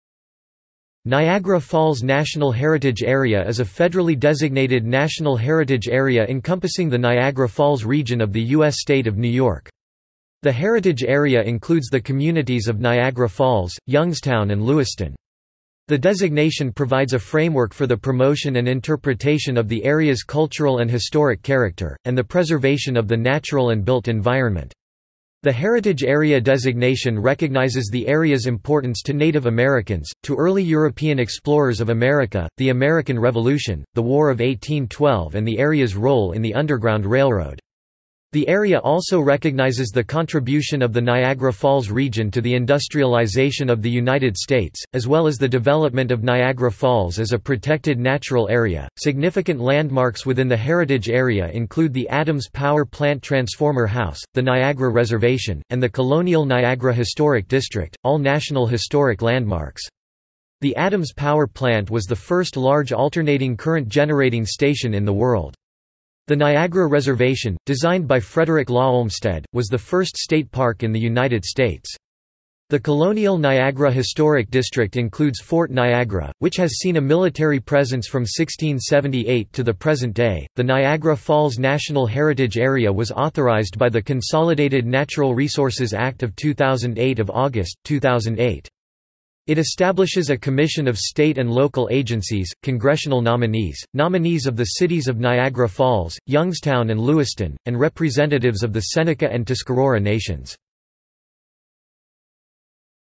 Niagara Falls National Heritage Area | Wikipedia audio article
- Socrates

SUMMARY
Niagara Falls National Heritage Area is a federally designated National Heritage Area encompassing the Niagara Falls region of the U.S. State of New York. The heritage area includes the communities of Niagara Falls, Youngstown and Lewiston. The designation provides a framework for the promotion and interpretation of the area's cultural and historic character, and the preservation of the natural and built environment. The heritage area designation recognizes the area's importance to Native Americans, to early European explorers of America, the American Revolution, the War of 1812 and the area's role in the Underground Railroad. The area also recognizes the contribution of the Niagara Falls region to the industrialization of the United States, as well as the development of Niagara Falls as a protected natural area.Significant landmarks within the heritage area include the Adams Power Plant Transformer House, the Niagara Reservation, and the Colonial Niagara Historic District, all National Historic Landmarks. The Adams Power Plant was the first large alternating current generating station in the world.  The Niagara Reservation, designed by Frederick Law Olmsted, was the first state park in the United States. The Colonial Niagara Historic District includes Fort Niagara, which has seen a military presence from 1678 to the present day.The Niagara Falls National Heritage Area was authorized by the Consolidated Natural Resources Act of 2008 of August, 2008. It establishes a commission of state and local agencies, Congressional nominees, nominees of the cities of Niagara Falls, Youngstown and Lewiston, and representatives of the Seneca and Tuscarora nations.